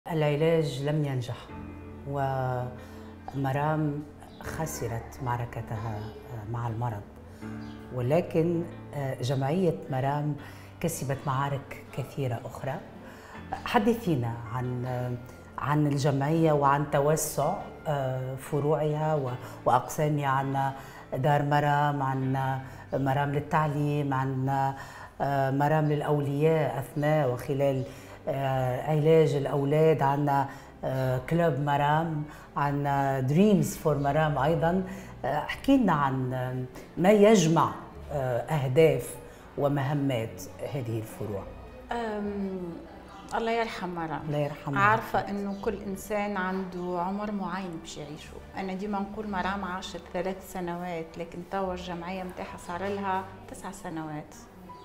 مرام خسرت معركتها مع المرض .. قصة تأسيس جمعية مرام لمرضى السرطان
منال الغربي امرأة تحدّت الموت بالحياة واليأس بالإيجابية، فاستطاعت أن تحوّل مأساة فقدان ابنتها مرام بمرض السرطان، أملاً لآلاف الأطفال المصابين بالمرض العضال. ابنتها توفيت، فأنجبت منال جمعية "مرام" التي لا تكتفي بالمساعدة على إيجاد العلاج بل تؤوي الأهل خلال مرحلة علاج أبنائهم، وتحقق أحلام الأطفال.